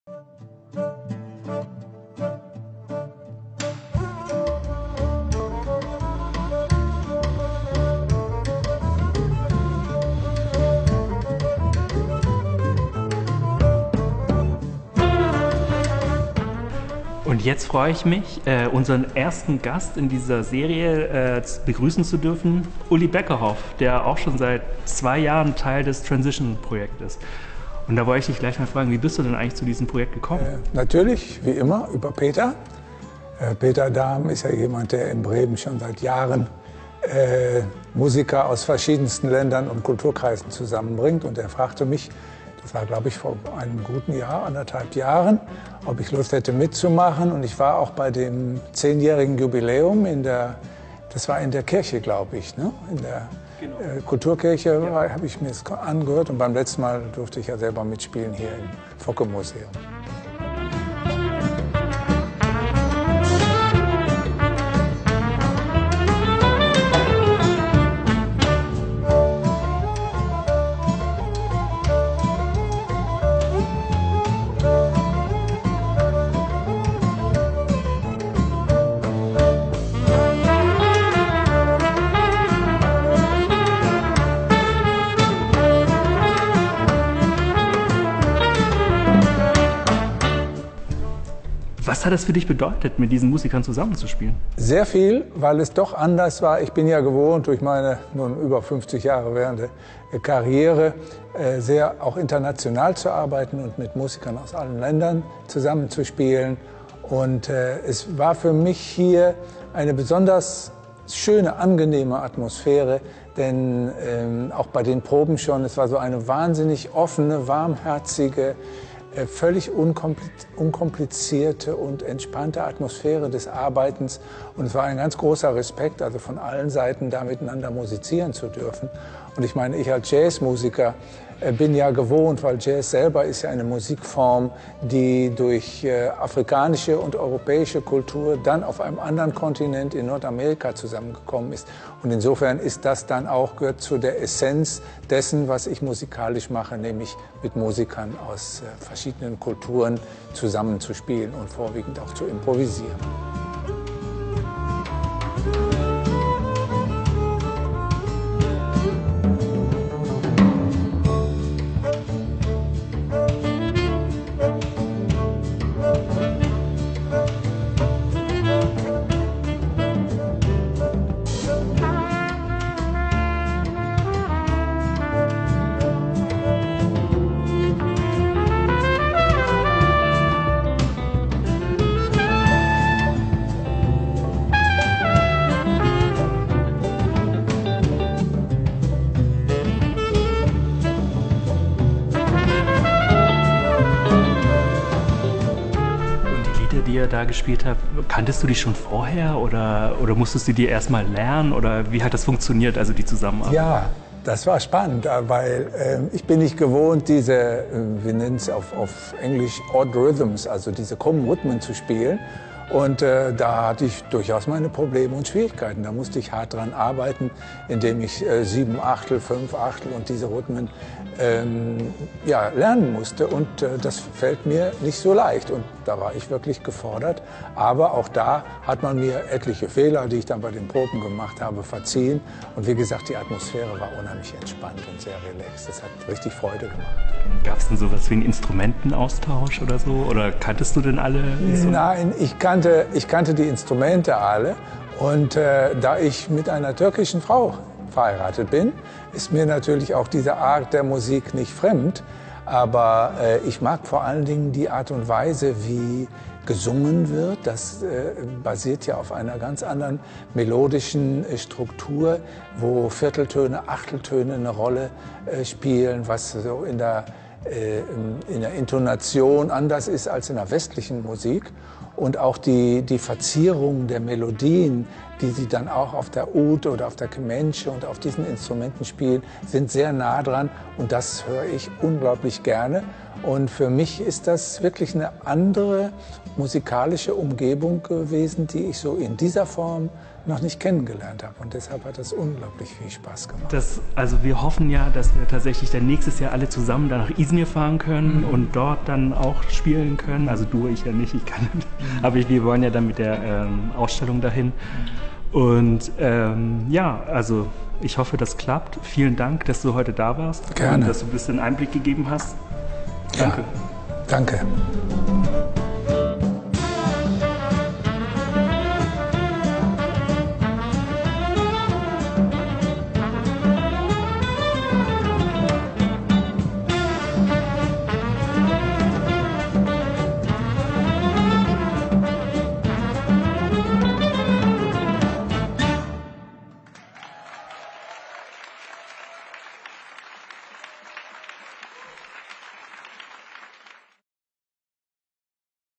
Transition Konseri Üzerine Bir Söyleşi | Bremen
Bremen’de, yerel yönetim (senato) destekli Westend Kültür Merkezi direktörü Peter Dahm genel sanat yönetmenliğinde ve Bremen şehir müzesi Focke Museum ev sahipliğinde Ekim 2019 gerçekleştirdiğimiz ve Almanya’nın en ünlü caz sanatçılarından Uli Beckerhoff ile aynı sahneyi paylaştığımız, Transition Projesi konseri üzerine; Focke Museum Direktörü Bora Akşen tarafından Beckerhoff ile yapılan söyleşi...